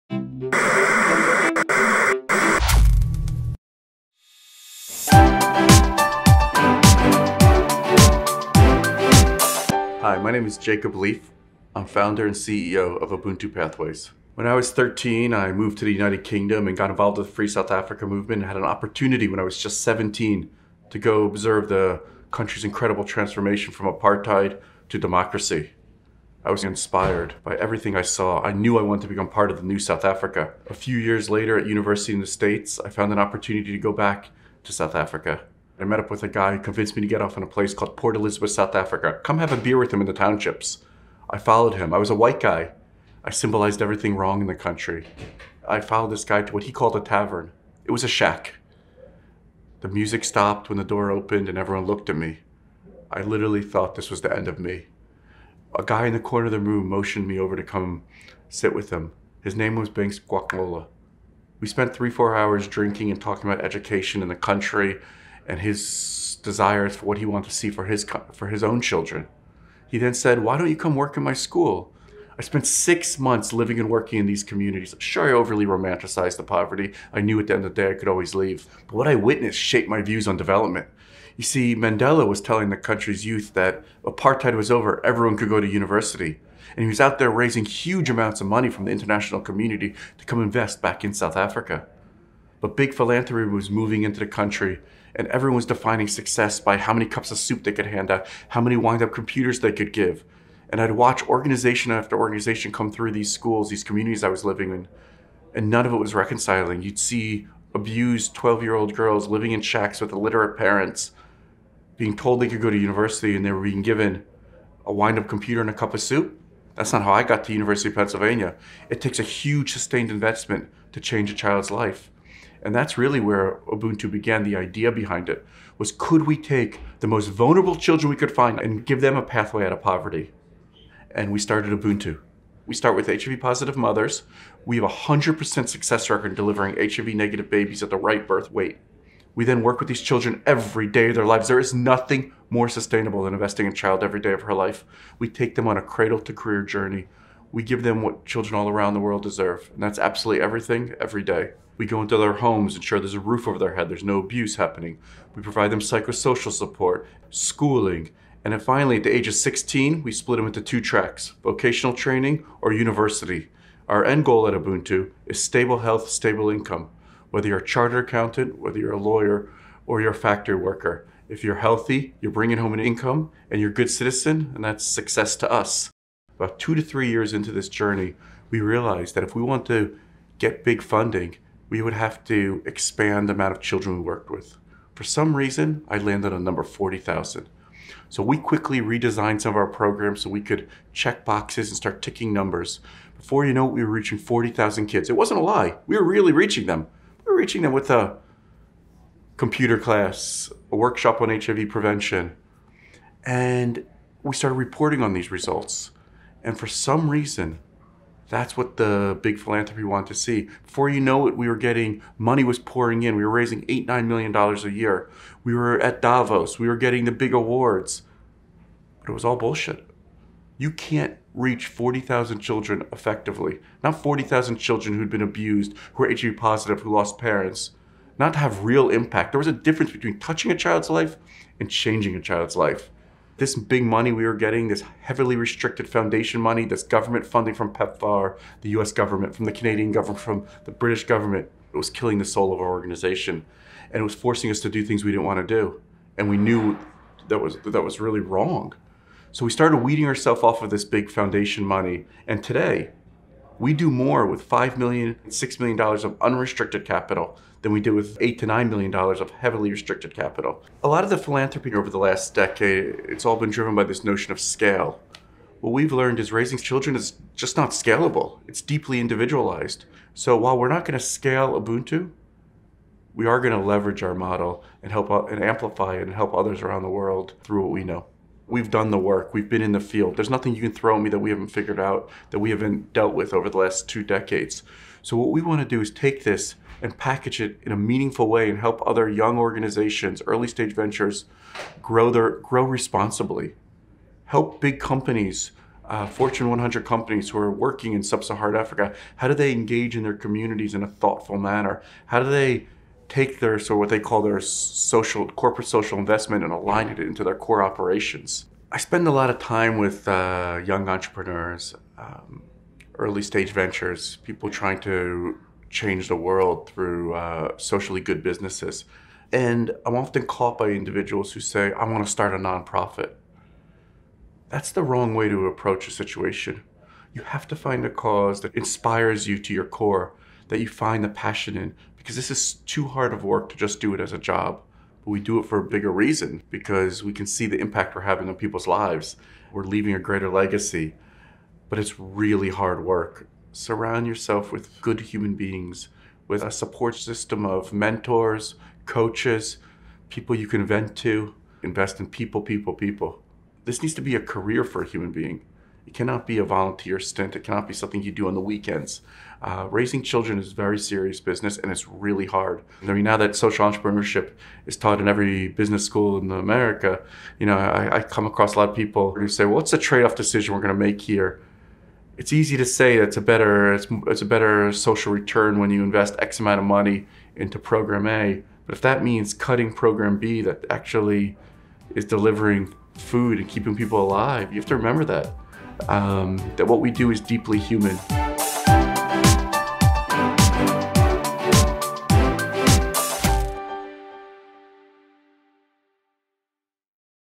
How a Nonprofit is Helping Thousands of African Children From Cradle to Career
Jessica Abo sits down with the founder and CEO of Ubuntu Pathways to learn how the nonprofit supports African children with individualized pathways out of poverty.